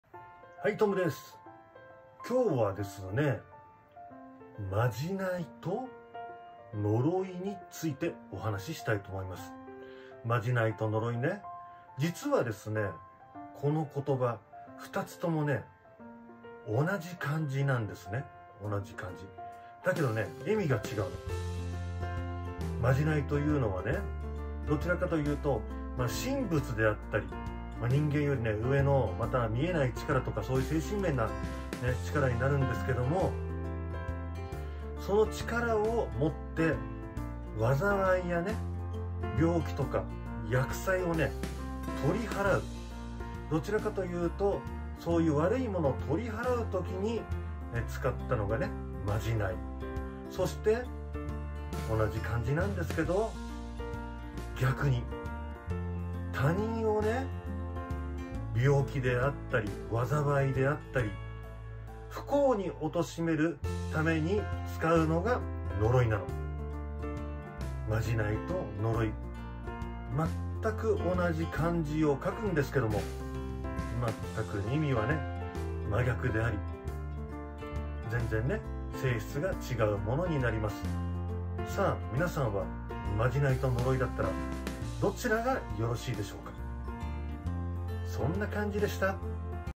開運雑学✨✨まじないと呪い🔯占い館luce トム先生
本日は『まじないと呪い』の違いについて。

同じ漢字ですが、意味が全く異なります。